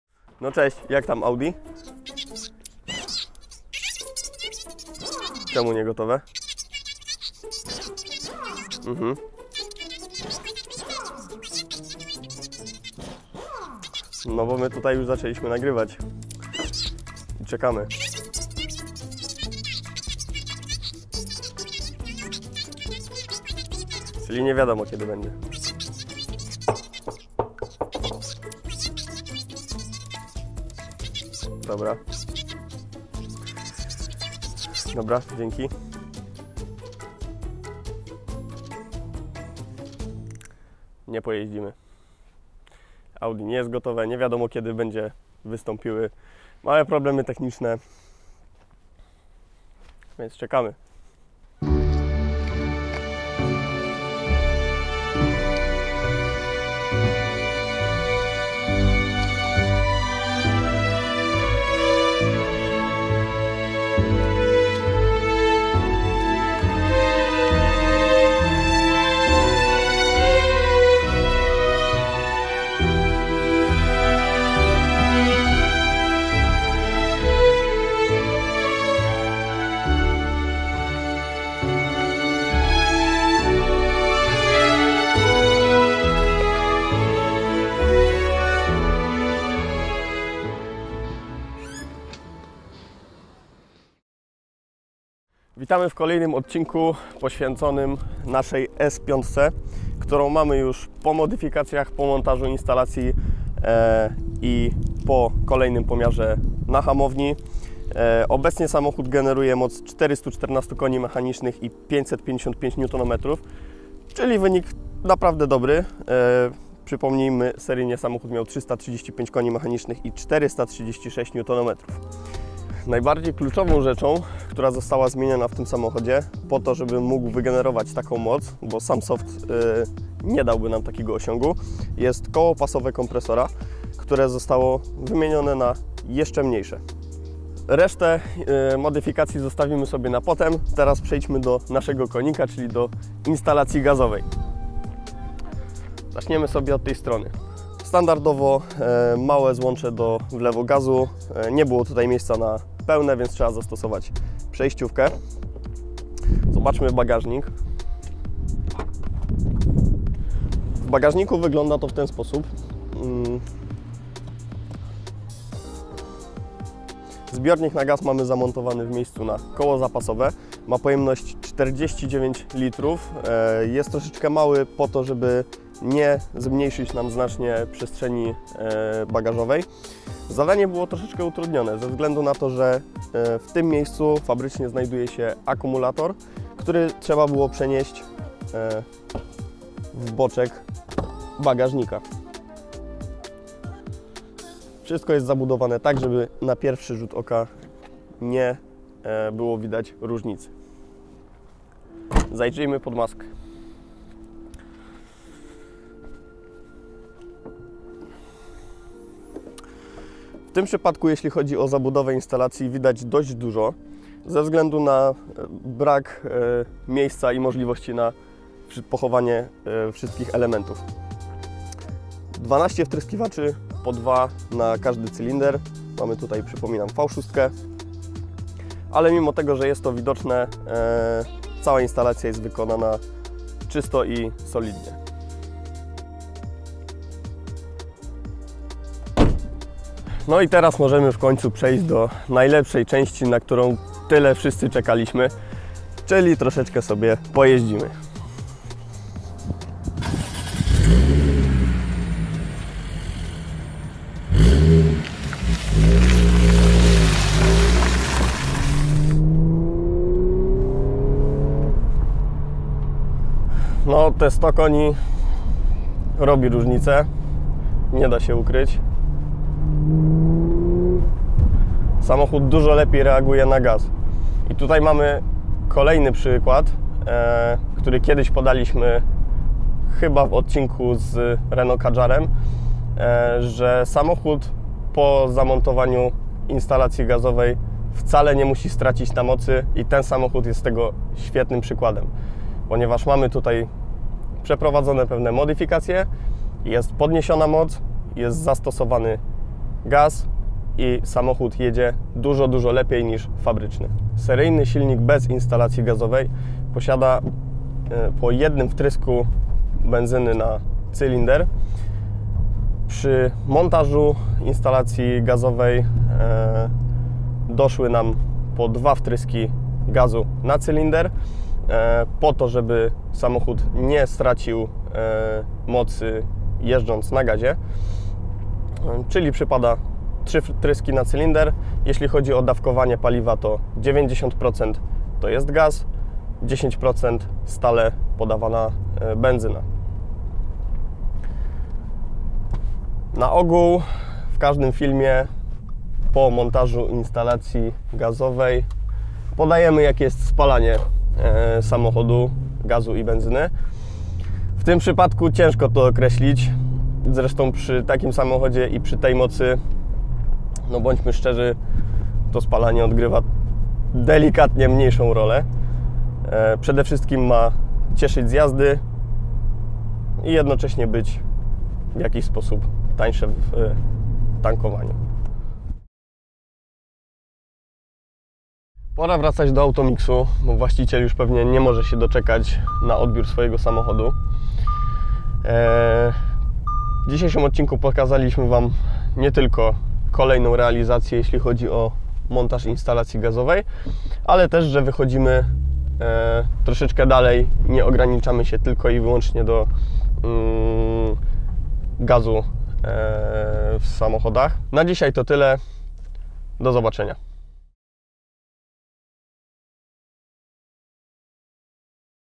Po tuningu montujemy instalacje LPG w Audi S5! 414 KM w gazie! Sprawdź efekty!
Sprawdź jaka cena instalacji LPG w Audi S5?

Audi S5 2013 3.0 V6 TURBO 333KM

Zamontowaliśmy instalację LPG w samochodzie Audi S5 z trzylitrowym silnikiem generującym moc 335 KM. Fabrycznie sześciocylindrowy silnik posiada po jednym wtrysku benzyny na cylinder. Dodaliśmy 12 wtryskiwaczy - po 2 na każdy cylinder. Zbiornik na gaz umieściliśmy w miejscu na koło zapasowe. Cała instalacja wykonana jest solidnie i estetycznie. Wprowadziliśmy również modyfikacje zwiększające moc silnika - obecnie jest to 414 KM. Samochód pobiera tylko 10% benzyny a 90% gazu, który jest o połowę tańszy od benzyny. Dzięki temu powstają znaczne oszczędności przy zakupie paliwa.